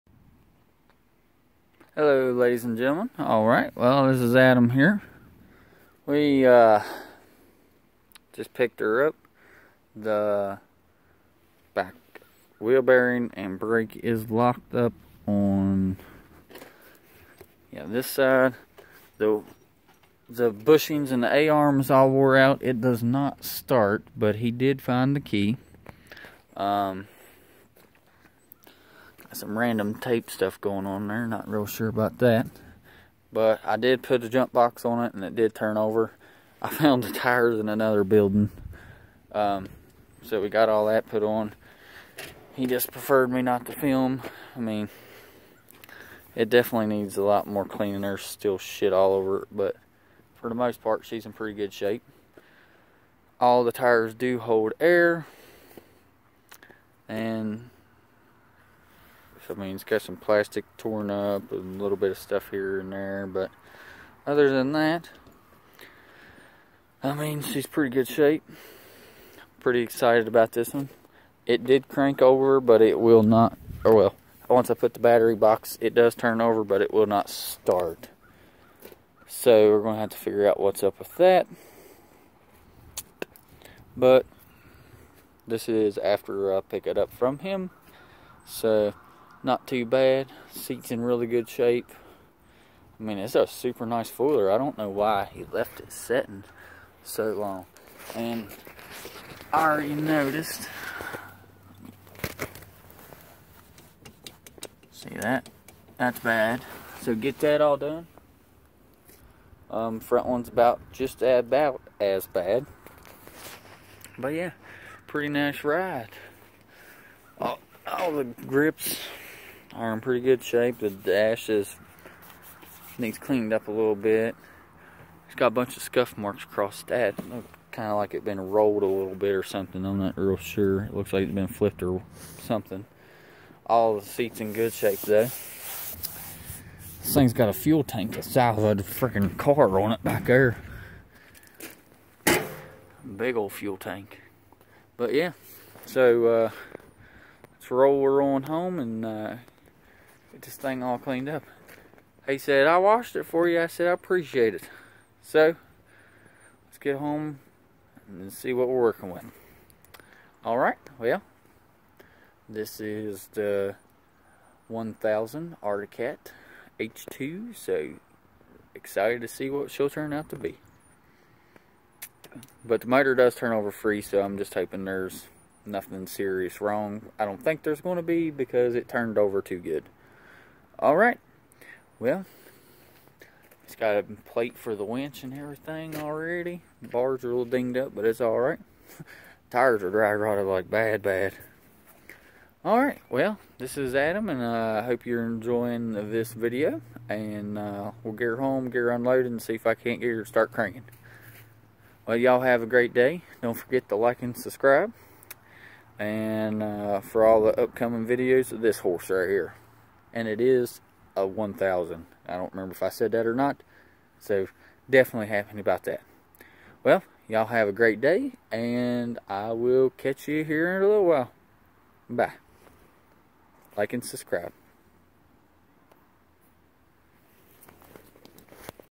2008 1000 Arctic Cat H2 was abandoned in the born
2008 Arctic Cat 1000 was abandoned in the barn walk around